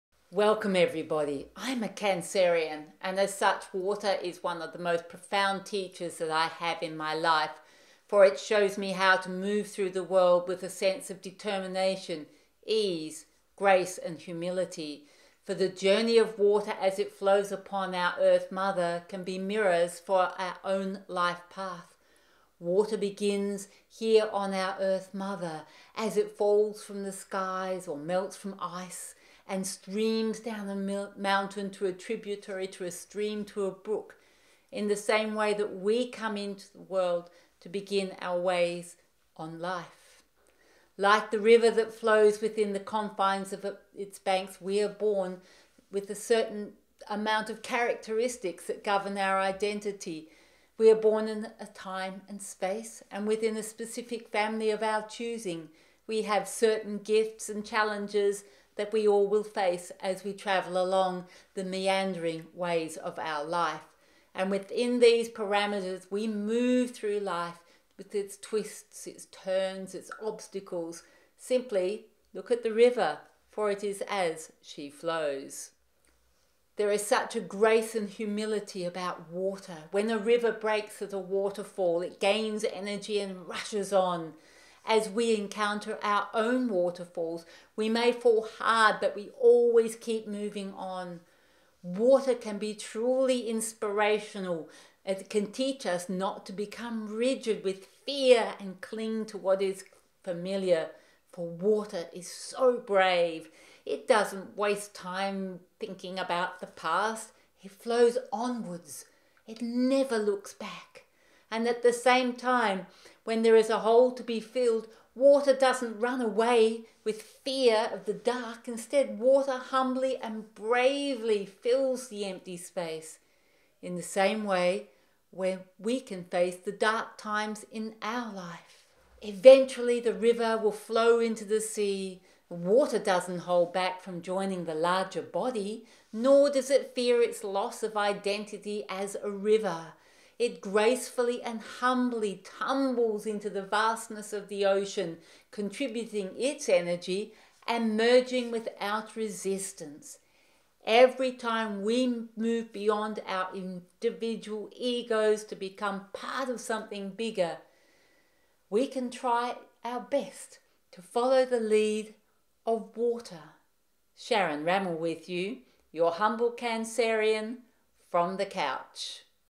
Water The River - From the Couch with Sharon Ramel
Water is my greatest teacher, born under the sign of Cancer it really show me the way.  Water gifts us and shows us how to move through the world with grace, ease, determination, and humility. When a river breaks at a waterfall, it gains energy and moves on, as we encounter our own waterfalls, we may fall hard but we always keep moving on. Water can inspire us to not become rigid with fear or cling to what's familiar. Water is brave and does not waste time clinging to its past, but flows onward without looking back.